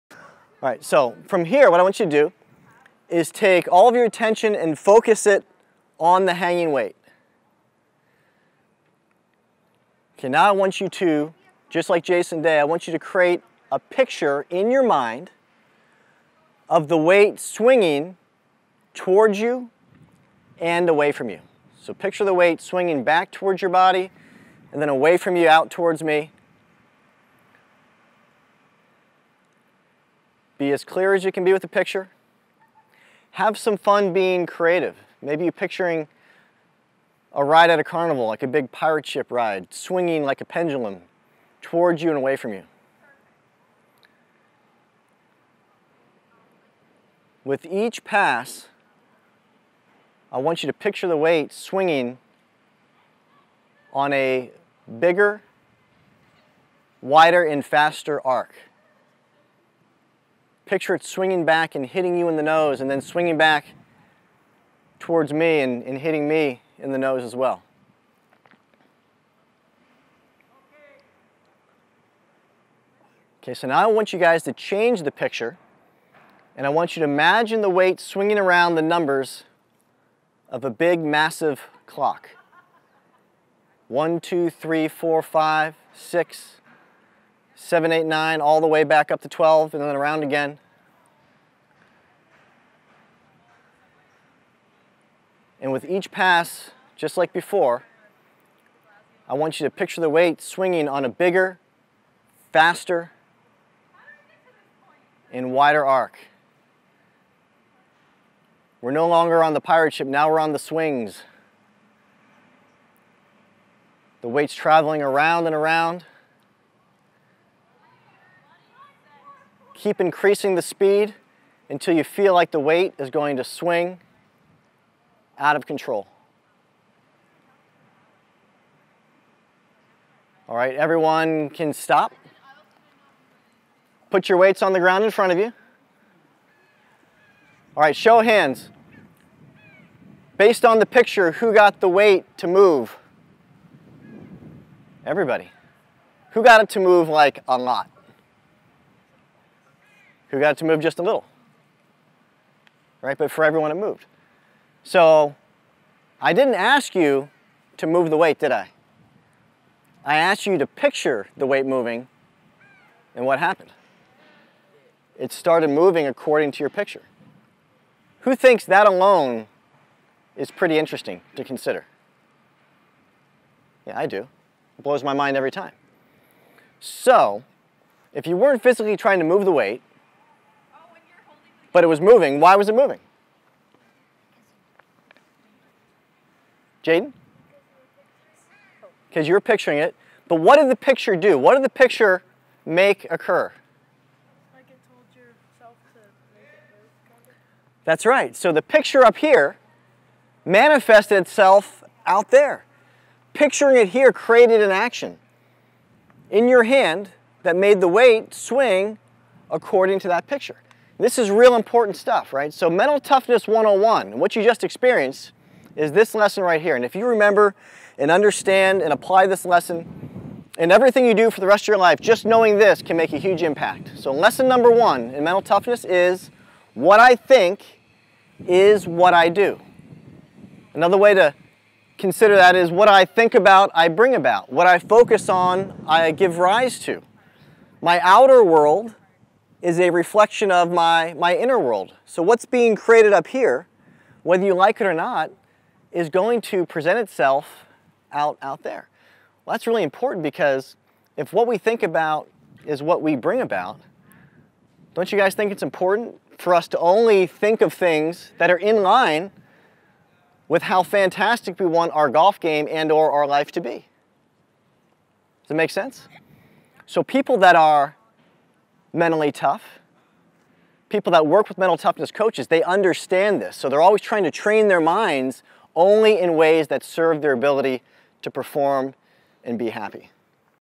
Mental Toughness Clinic Part 1 - Imagery
Jeff Ritter shares an exercise in mental imagery to help create inspired action on and off the golf course.